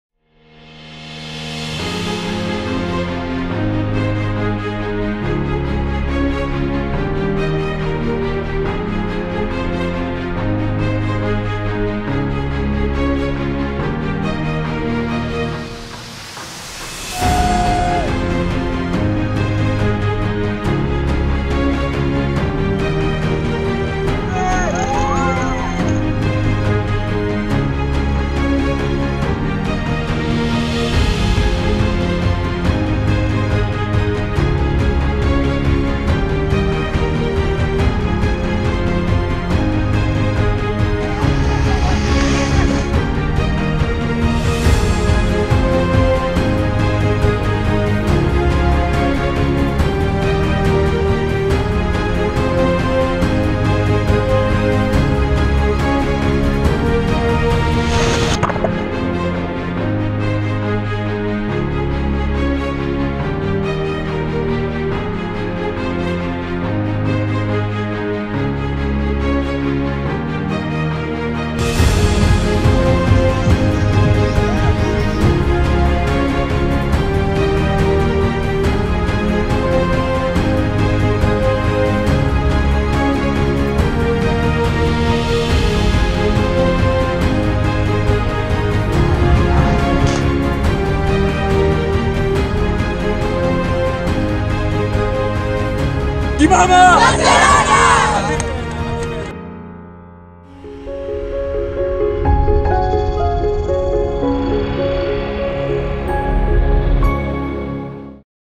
Kimama Barcelona on GoPro
We gave campers and counselors at Kimama Barcelona 2017 a GoPro camera and then edited the footage. This is what came out.